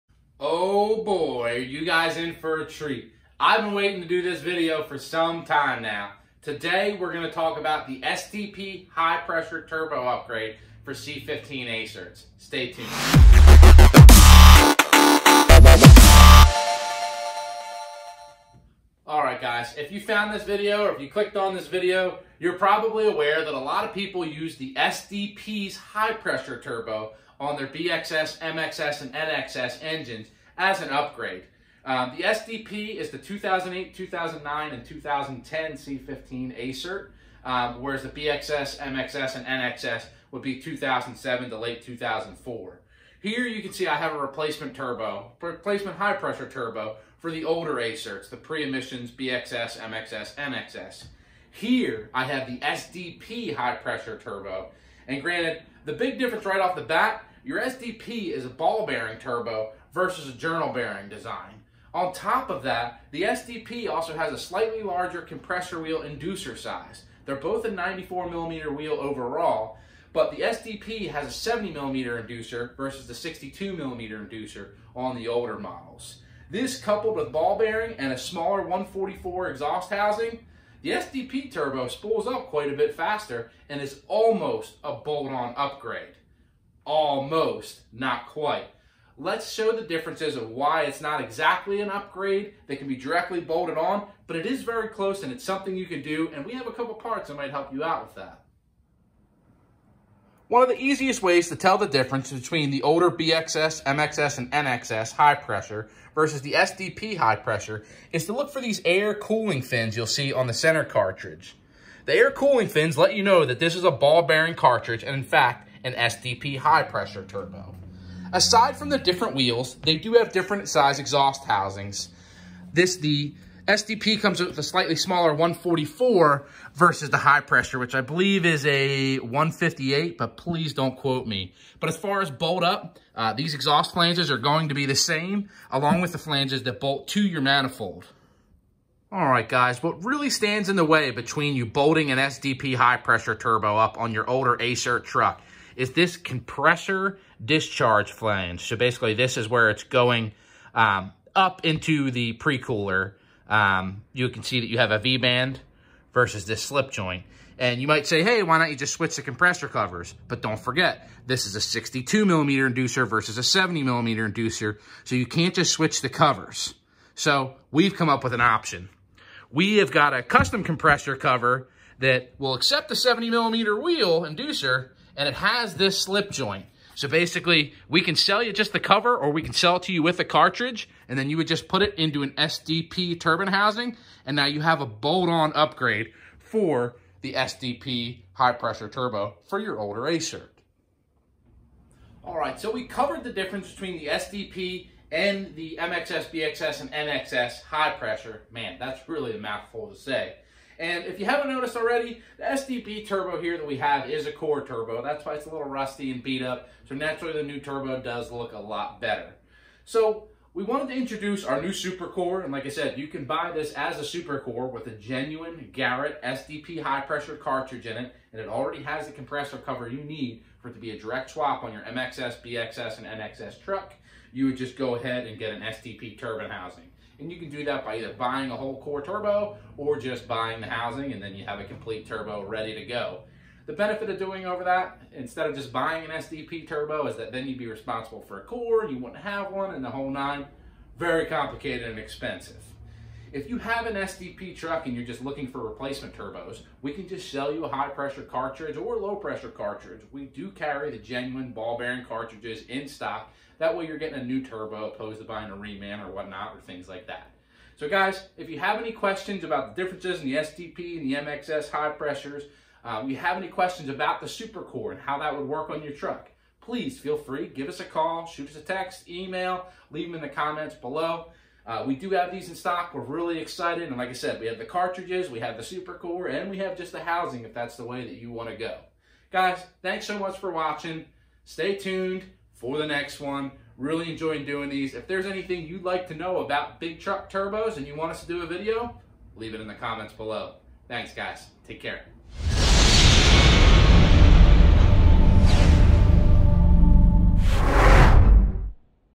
Ball Bearing SDP Upgraded High Pressure Turbo for Cat C15 Acert
If you're looking to upgrade the factory high pressure turbo on your BXS, MXS, or NXS C15 acert, the SDP is a great option!